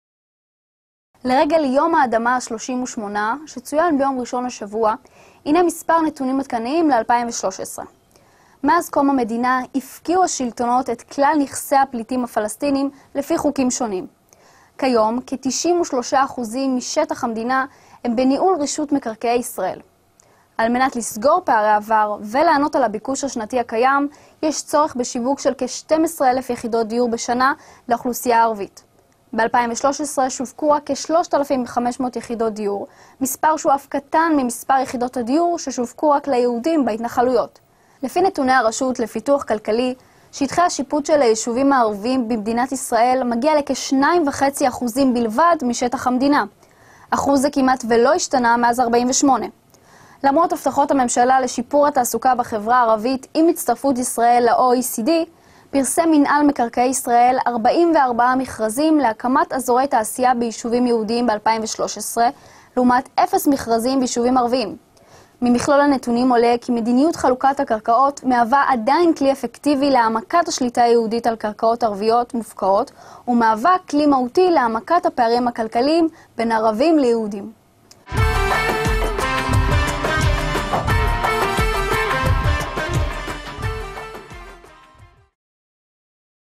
Land Day 38
Since Israel's establishment, authorities have confiscated assets of Palestinian refugees using various regulations. Today, about 93% of the country is under Israel Lands Authority. To close social gaps and meet yearly demands, it's necessary to marketing 12,000 housing units per year for the Arab population. In 2013 only 3,500 units were sold, a number smaller than housing units sold to Jews in the settlements. According to the Economic Development Authority, the area of Arab communities in Israel is about 2.5% of the country's total area. This percentage has not changed since '48. Despite government promises to improve employment in the Arab sector following Israel's joining the OECD, Israel published forty-four tenders for the construction of industrial areas in Jewish communities in 2013, compared with zero tenders in Arab communities. Data indicate that land distribution policy is still an effective tool for expanding Jewish control over expropriated Arab lands, and is an essential tool for deepening economic disparities between Arabs and Jews.